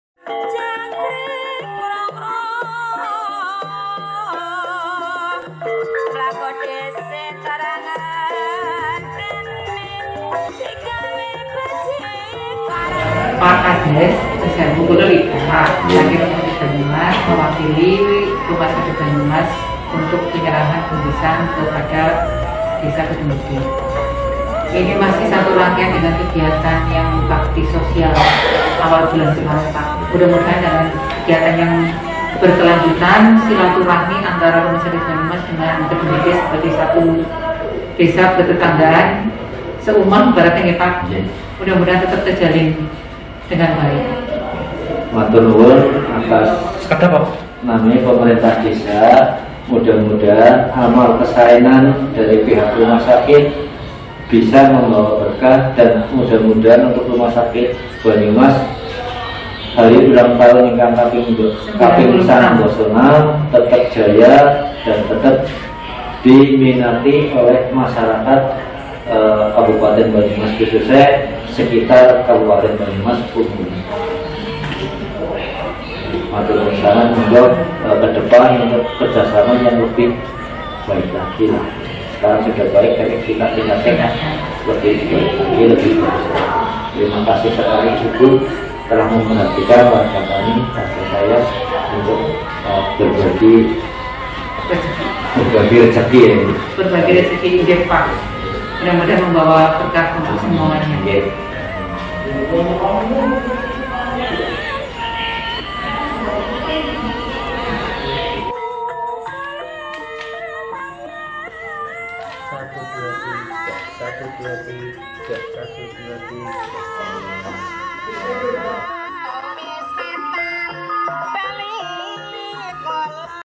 RSUD Banyumas Bagi Sembako di Desa Kedunggede
BANYUMAS - Hari ini Jumat (07/05/2021) Dalam Rangkaian HUT RSUD Banyumas yang ke-96, Jajaran Management RSUD Banyumas, Panitia HUT RSUD Banyumas dan Bina Rohani Islam RSUD Banyumas serta Instalasi P2CSR menyelenggarakan kegiatan Bakti Sosial dengan membagi Paket sembako ke Desa Binaan RSUD Banyumas,yaitu Desa kejawar dan Desa Kedunggede Banyumas.

Paket Sembako yang kita bagikan hari ini sejumlah 80 paket, untuk kita bagi 2 desa tsb. Masing masing mendapat 40 buah Paket.
Untuk Distribusi Paket sembako untuk warga desa di serahkan ke Kepala Desa Kedunggede dan Kepala Desa Kejawar.

Kepala Instalasi P2CSR RSUD Banyumas sdr. Lita Widiarti,S.Kep.,Ns menyerahkan Paket sembako kepada Ke-2 Kepala Desa Binaan RSUD Banyumas.
Semoga dengan terselenggarakan Kegiatan ini,Hubungan kita dengan ke-2 Desa Binaan RSUD Banyumas semakin mesra.

Salam Sehat
Salam Sejahtera.
Patuhi Protokol Kesehatan dengan Gerakan 5 M : Memakai Masker,Mencuci Tangan,Menjaga Jarak,Menghindari Kerumunan dan Membatasi Mobilitas.
Sukses untuk Kita Semua.